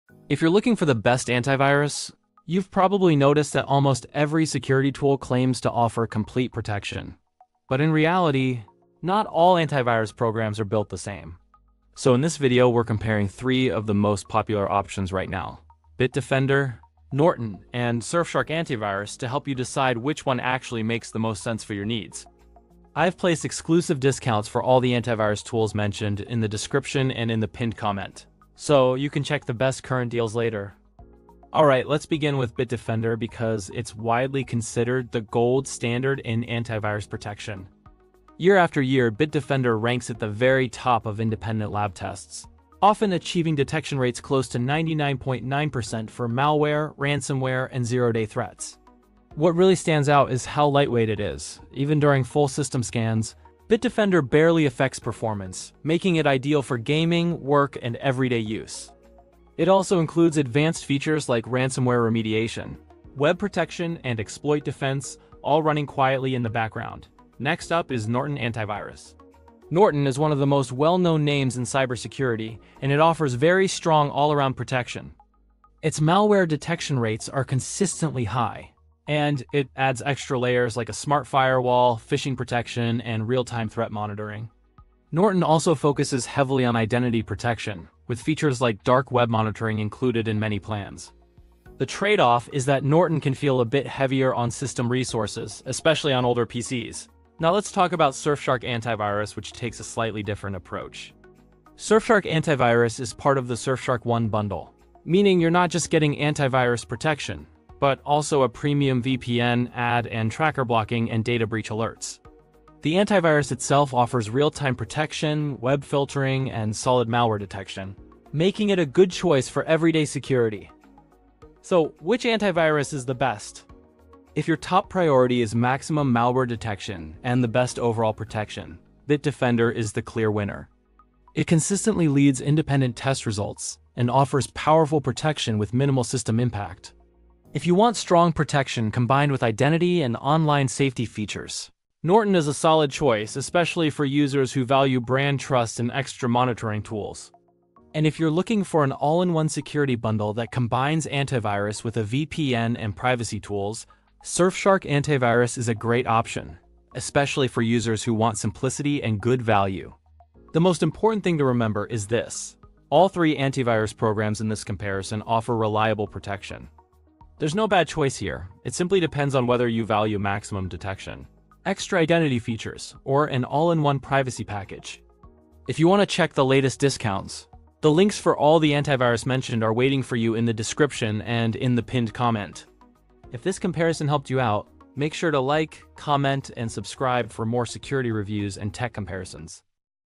Best Antivirus 2026 – Top Security Software Tested & Ranked
In this video, I rank the best antivirus software of 2026 after real testing. I compare malware protection, system impact, features, and overall value to see which security tools actually keep you safe. If you’re looking for reliable protection for PC, Mac, or mobile, this guide helps you choose the right antivirus without the hype.